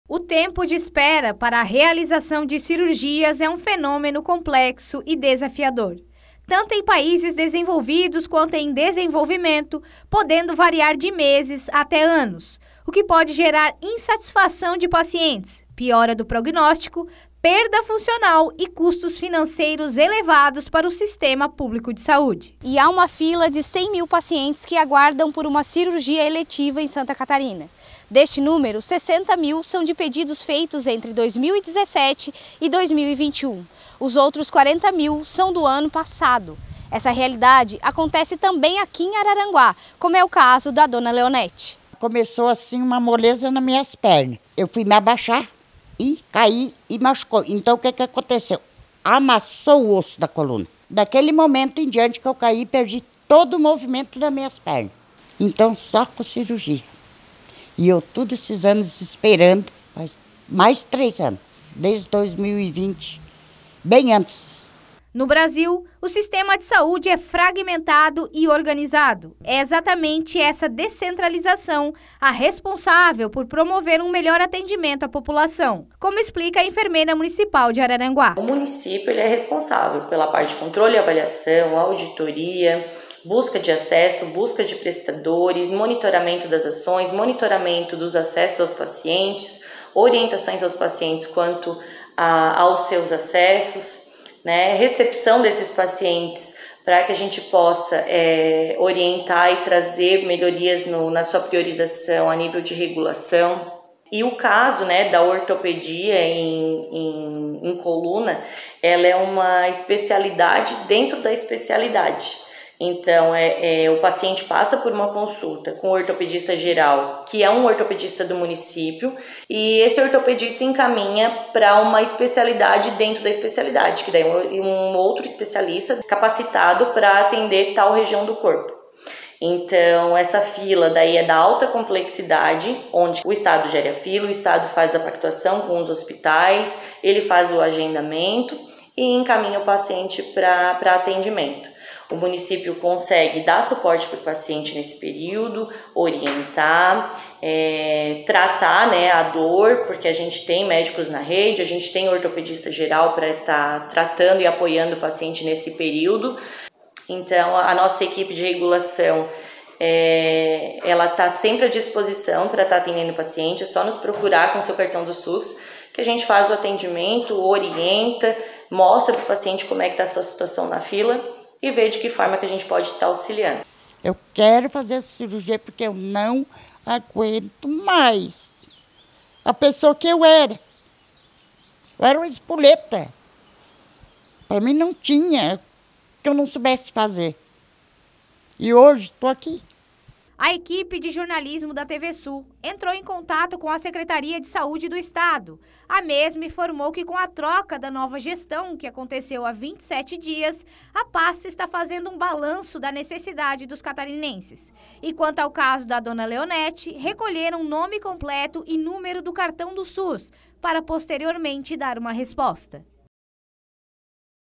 27/01/23 MORADORA DE ARARANGUÁ AGUARDA HÁ 3 ANOS POR CIRURGIA NA COLUNA
Reportagem: Manuela Oliveira
Data: 27/01/23